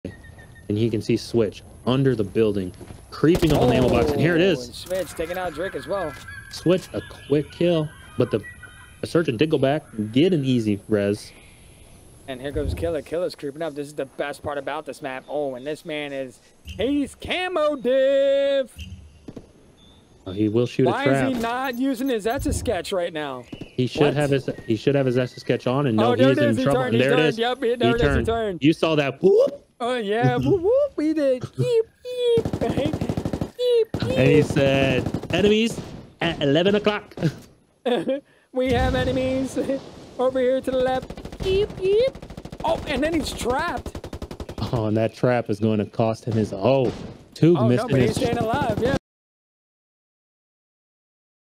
Echelon In Jungle Base?
Ling & Rat catch a Echelon bush whispering in it's natural habitat that is Jungle Base during the 100 $hmoney Goon Squad Sponsored Tournament!